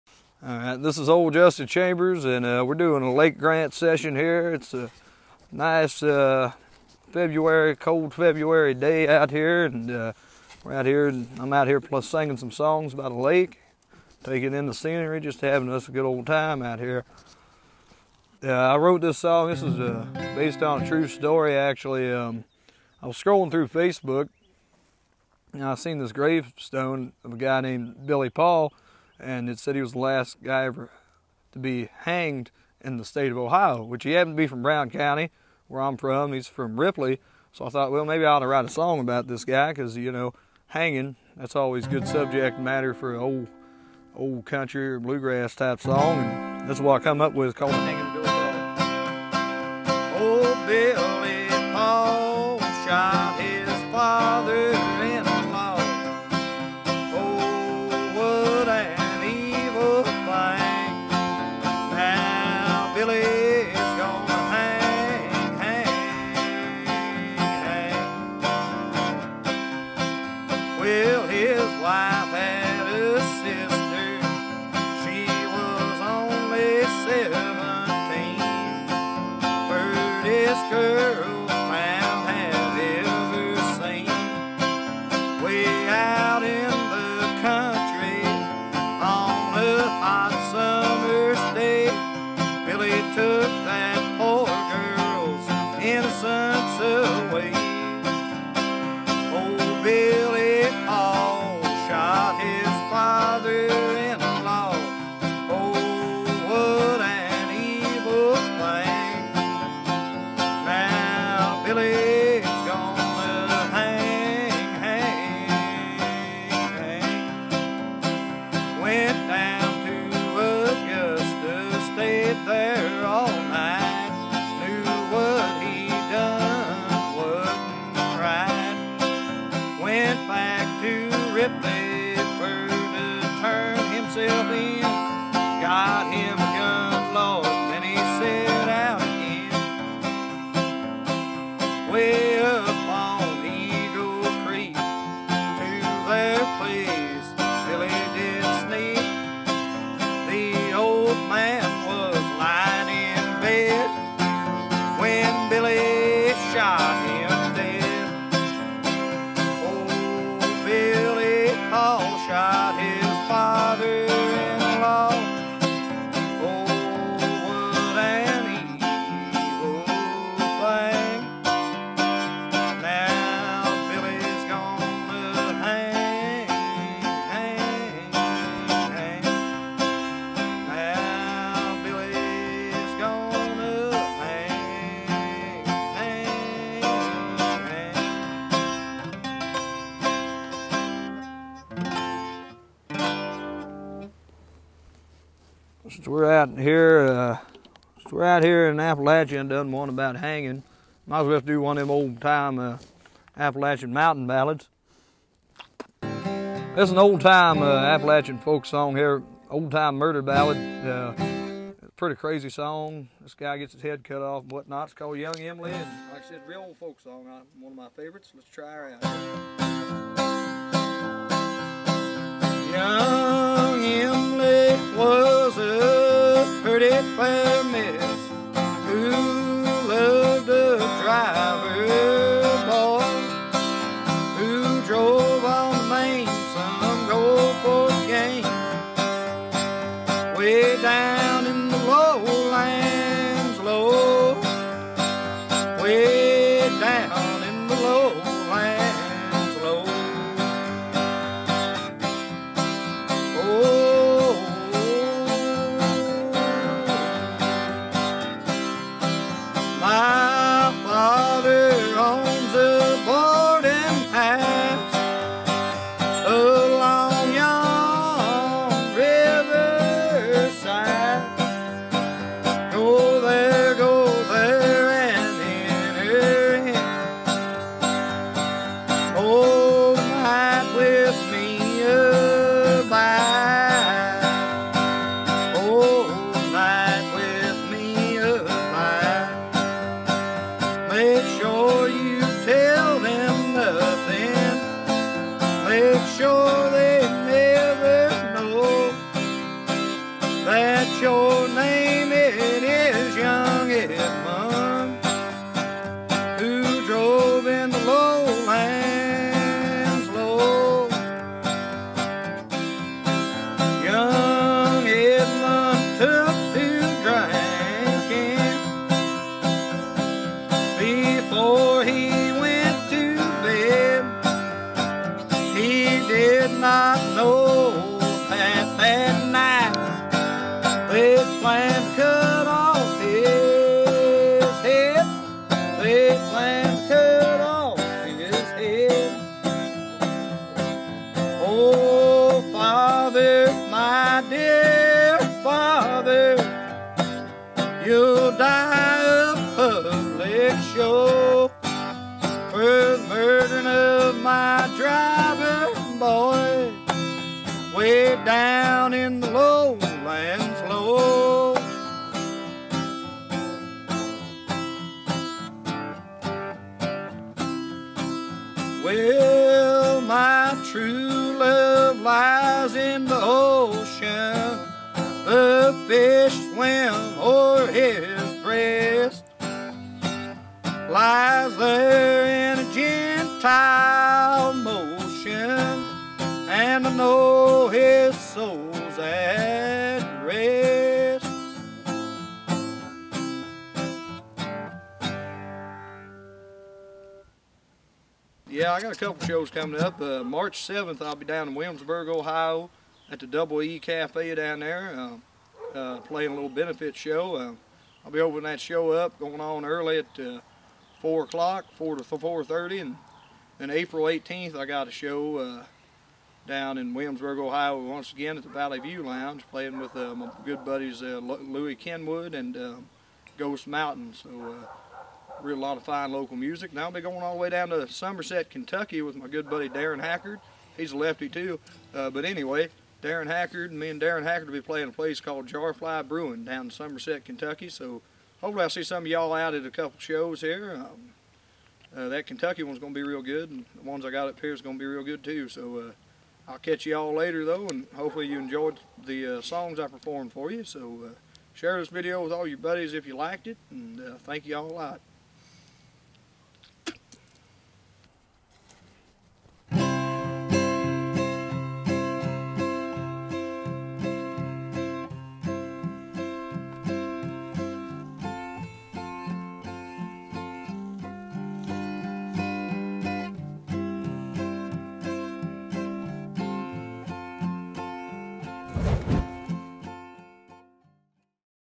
Lake Grant Sessions - Ole Justin Chambers
Southwest Ohio, songwriter and musician, Ole Justin Chambers performs two tunes to kick off Lake Grant Sessions.